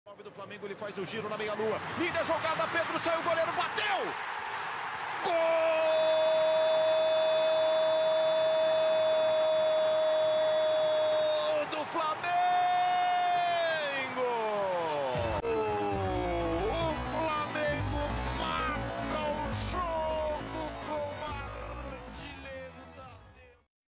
Pedro faz gol absurdo e manda recado para Sampaoli!😨
Flamengo x coritiba campeonato Brasileiro 2023
Flamengo 3 X 0 coritiba pelo campeonato Brasileiro.